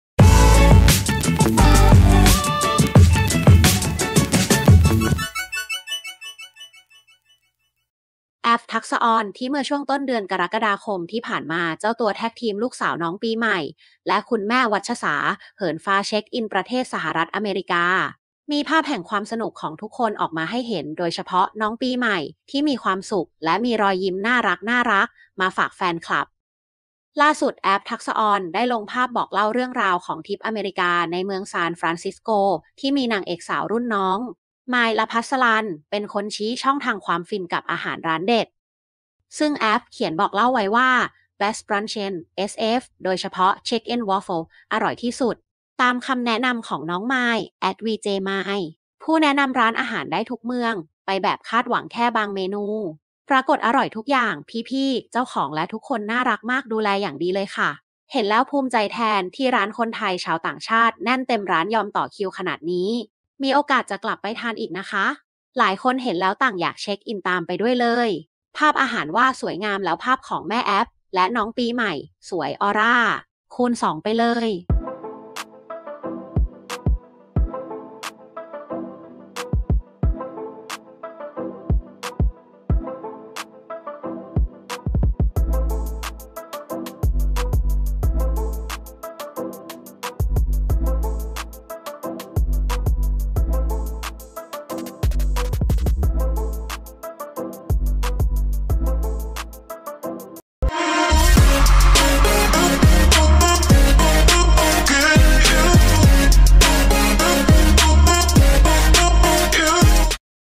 ภาพแห่งความสุข "แอฟ&น้องปีใหม่" แนะร้านเด็ด! หลายคนต่างอยากเช็กอินตาม
ขอบคุณภาพและคลิป